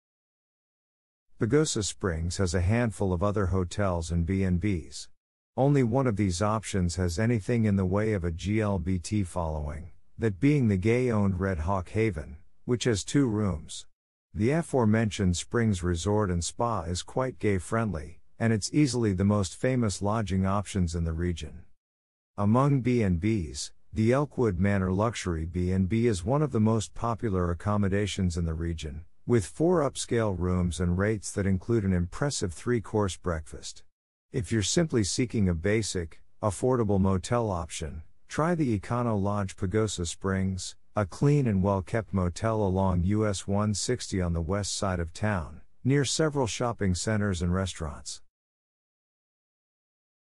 Pagosa Springs Hotel and BandB Guide
Pagosa Springs has a handful of other hotels and B&Bs. Only one of these options has anything in the way of a GLBT following, that being the gay-owned Red Hawk Haven, which has two rooms. The aforementioned Springs Resort & Spa is quite gay-friendly, and it's easily the most famous lodging options in the region..
Among B&Bs, the Elkwood Manor Luxury B&B is one of the most popular accommodations in the region, with four upscale rooms and rates that include an impressive three-course breakfast. If you're simply seeking a basic, affordable motel option, try the Econo Lodge Pagosa Springs, a clean and well-kept motel along U.S. 160 on the west side of town, near several shopping centers and restaurants..